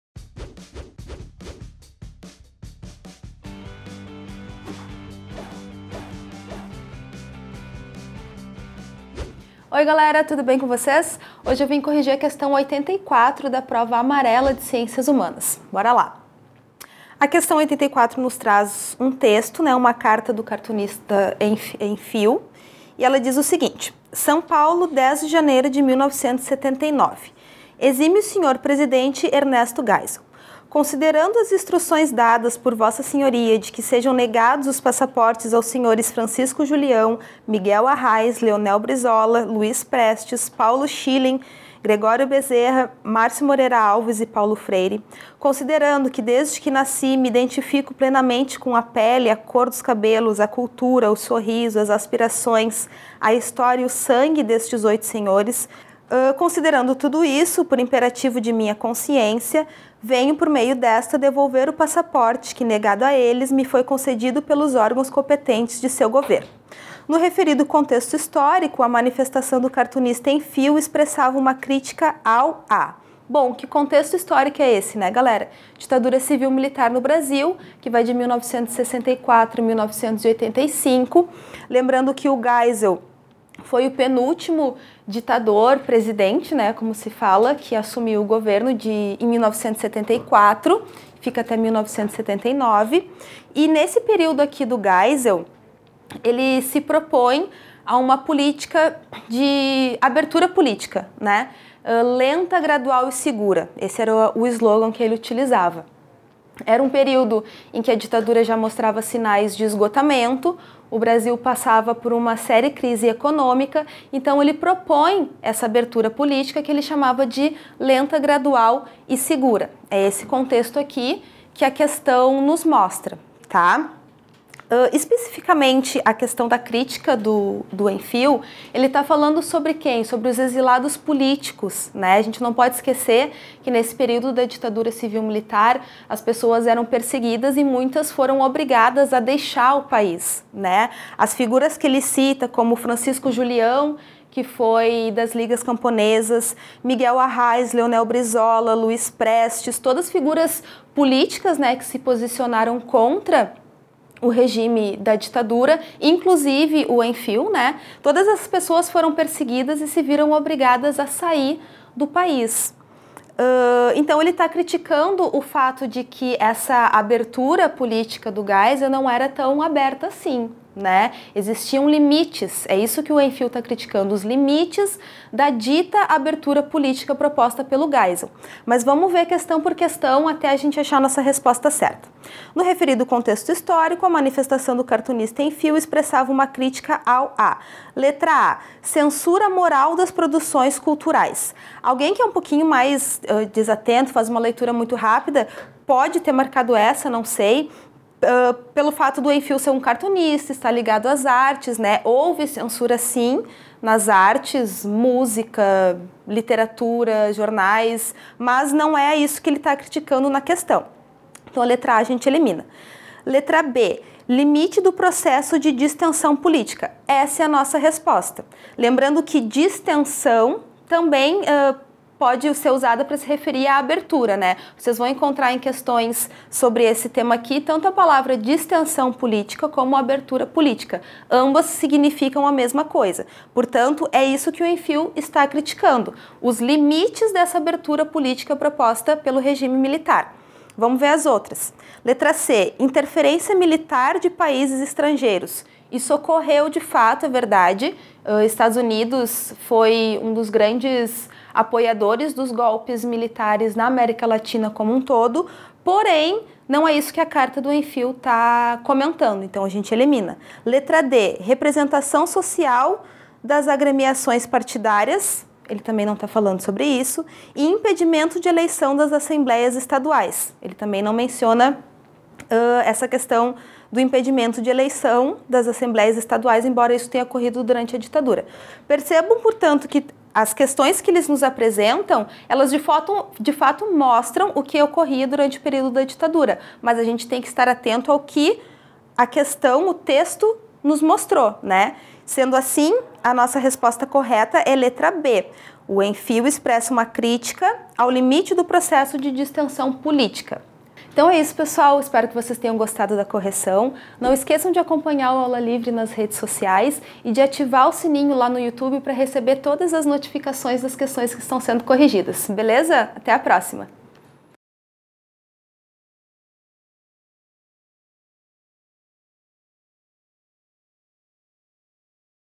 ENEM 2018 | Exílio e Ditadura | questão 84 (caderno amarelo)
Na questão 84 de História do Brasil do caderno amarelo de ciências humanas é preciso ter conhecimento sobre o exílio no período da Ditadura Civil-Militar.

Gostou desse vídeo? Então acesse nosso site e fique ainda mais preparado com nosso curso completo: